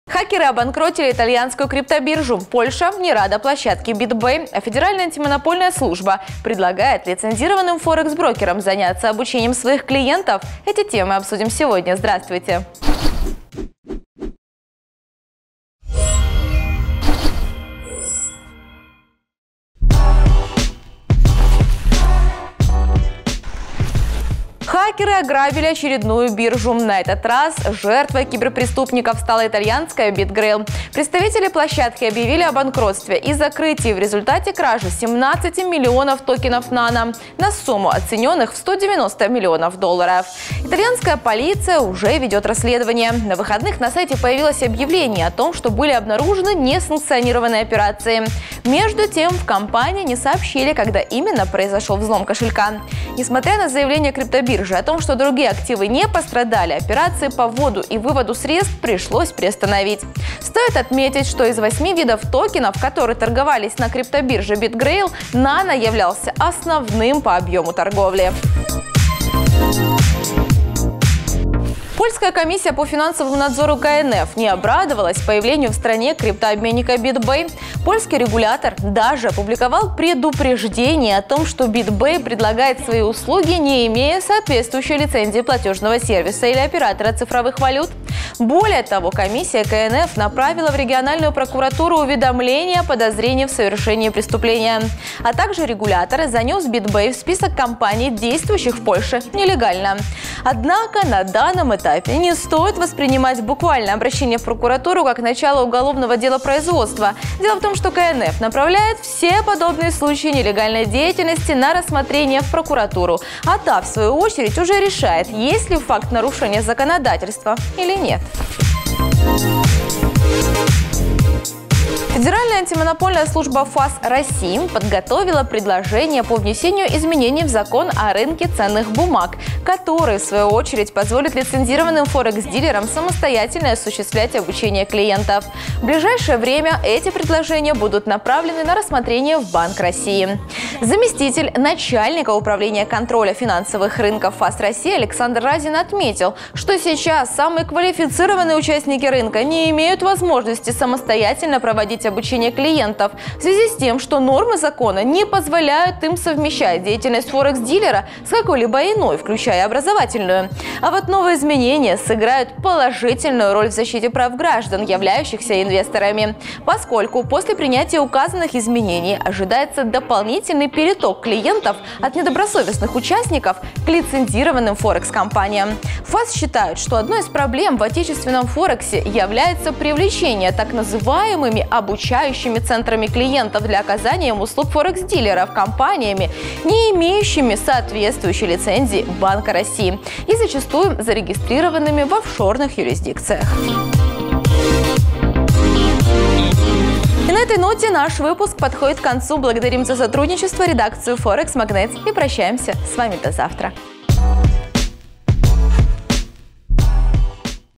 Хакеры взломали BitGrail, Польша давит на BitBay, Закон "О рынке ценных бумаг" РФ
Хакеры обанкротили очередную криптобиржу
В Польше не рады BitBay
ФАС хочет разрешить обучение клиентов